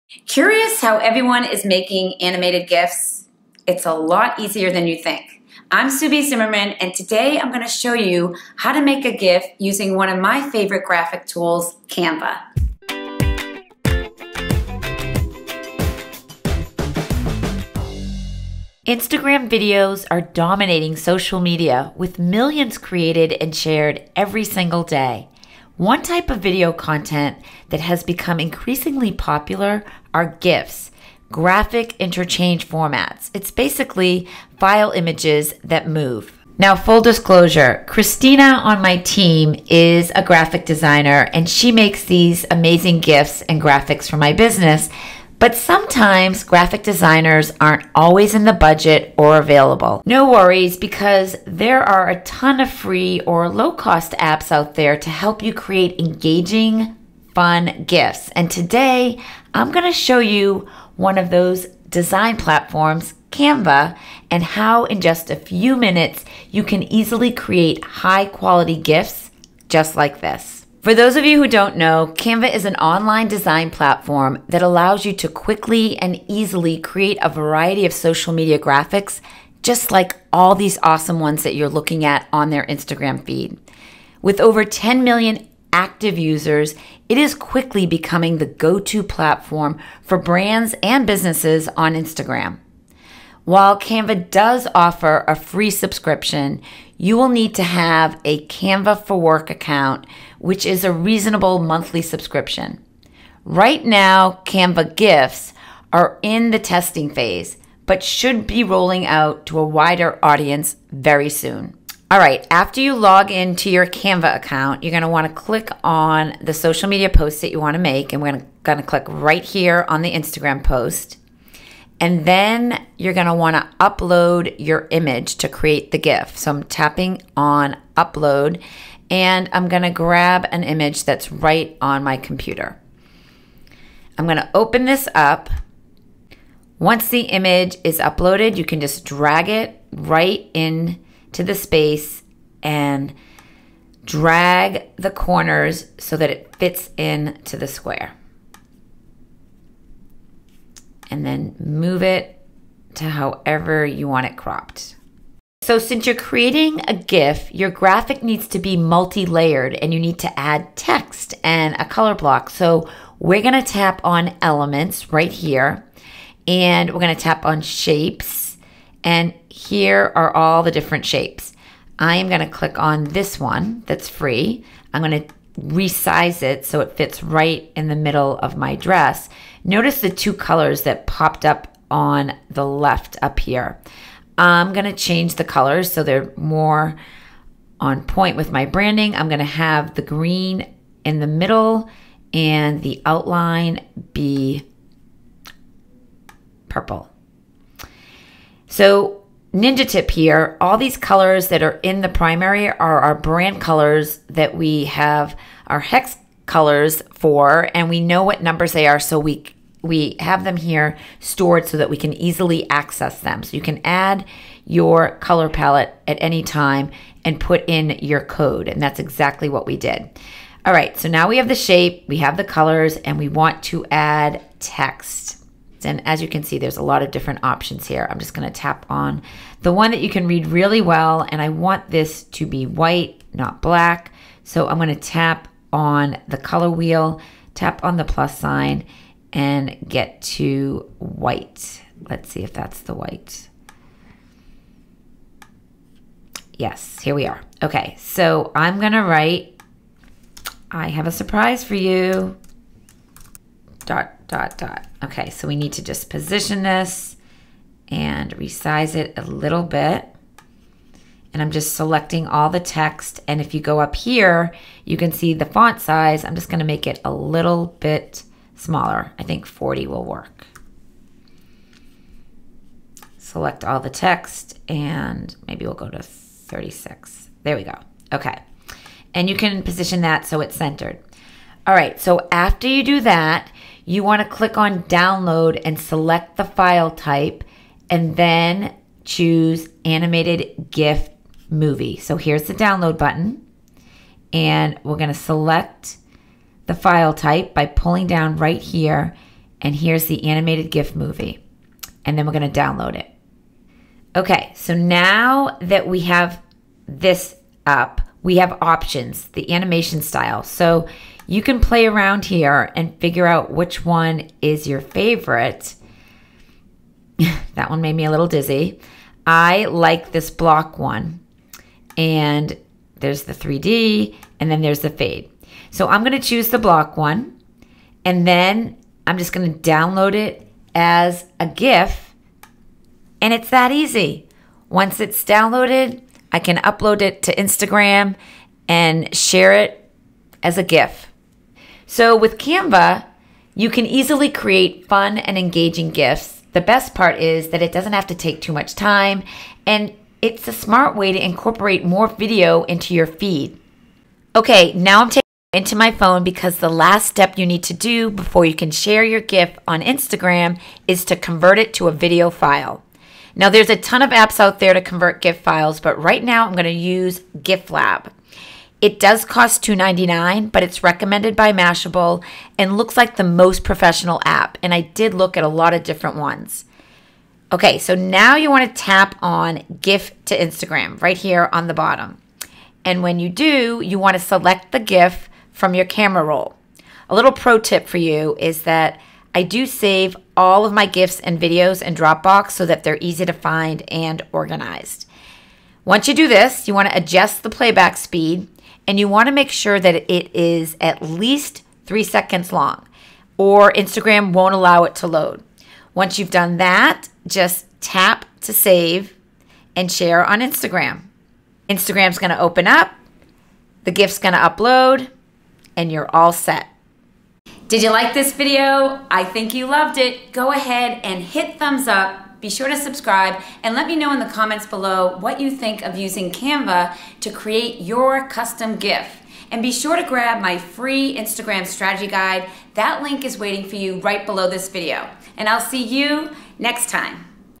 How to Create a GIF for Instagram Using Canva
How do you make a GIF?
Free training: The 3 Keys To Getting Highly Engaged Followers and Buyers...without getting overwhelmed by complex content creation

While there are a ton of free apps and websites out there, my favorite way to create GIFs is with Canva. Check out how in just a few minutes Canva can help you create engaging, high-quality GIFs for your brand.